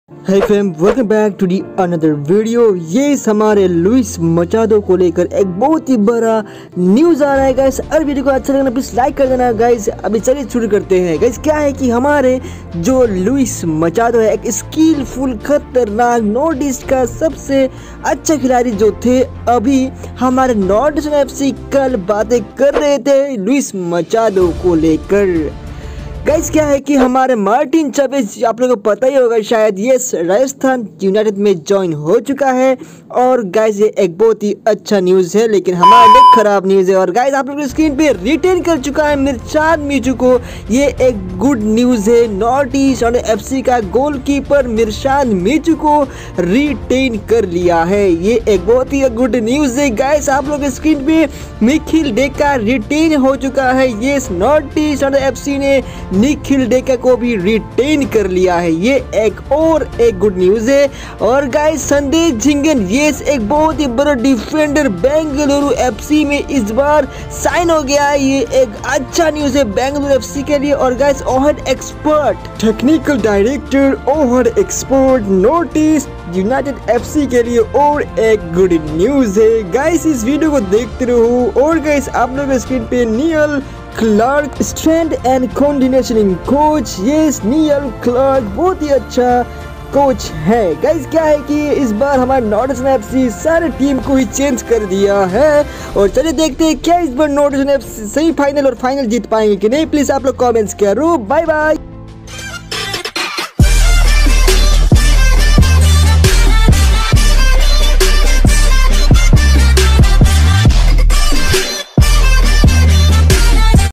NORTHEAST UNITED FC 😍 Luis Machado, Martin Chaves,Return😍Machado,Northeast All Coaching staff Change
india vs afghanistan highlights,
sunil chhetri,
Hey Everyone Whatsupp...  Welcome To ALLUPDATE Family ☺